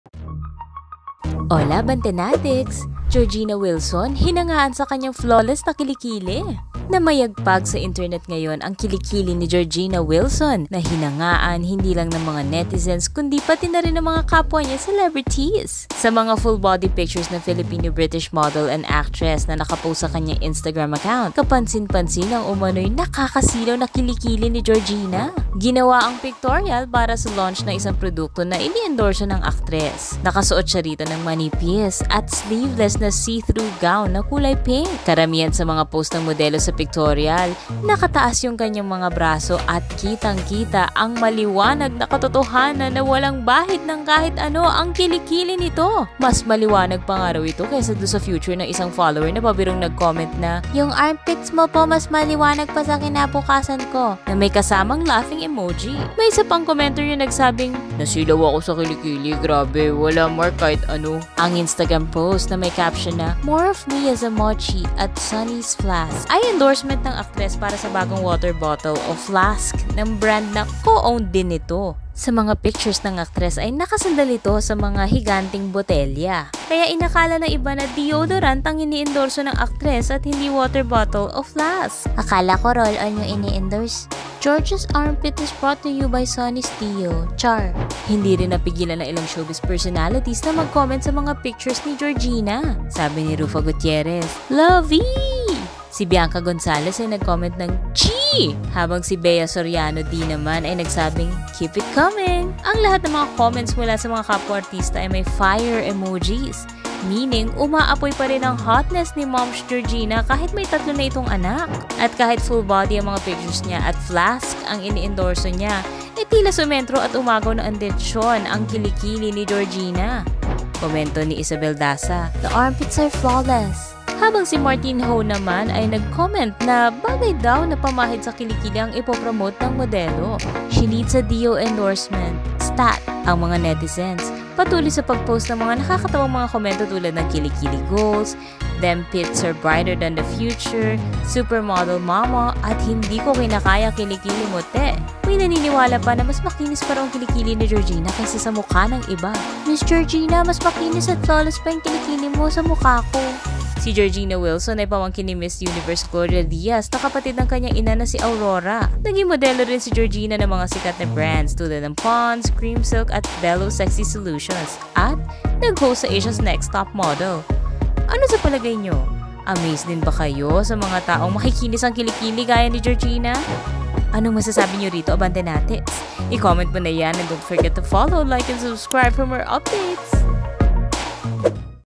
Georgina Wilson, hinangaan sa “Flawless” na kili kili
Kili-kili ni Georgina Wilson, viral ngayon sa internet!